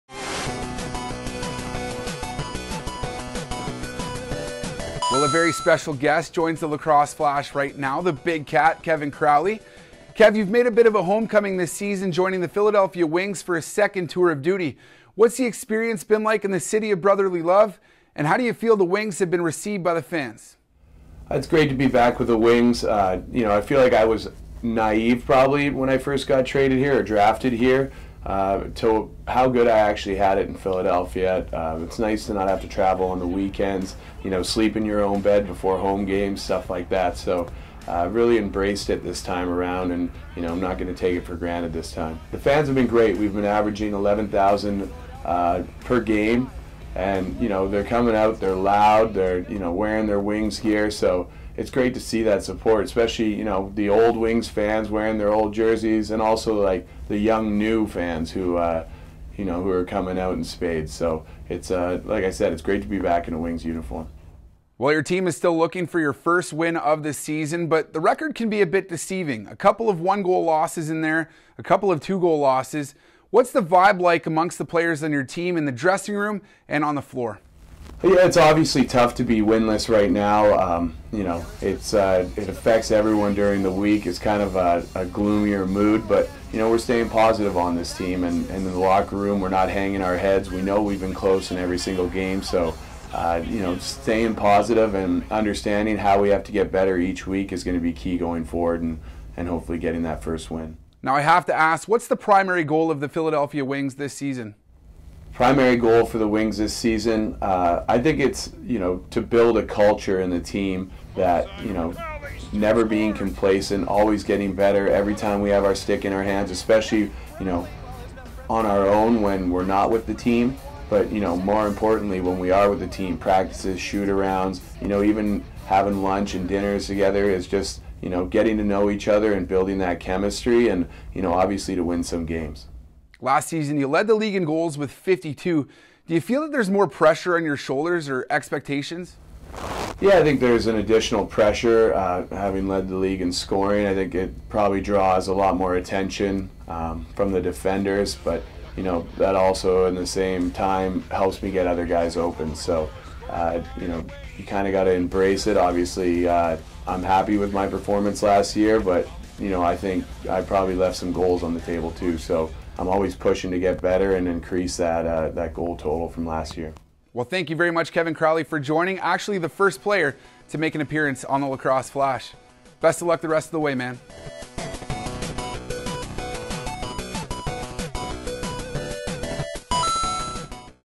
Lacrosse Flash: Philadelphia Wings' Kevin Crowley
Kevin Crowley of the Philadelphia Wings joins Tyson Geick of the Lacrosse Flash to talk about his "homecoming" to Philadelphia, the excitement of being back, and much more.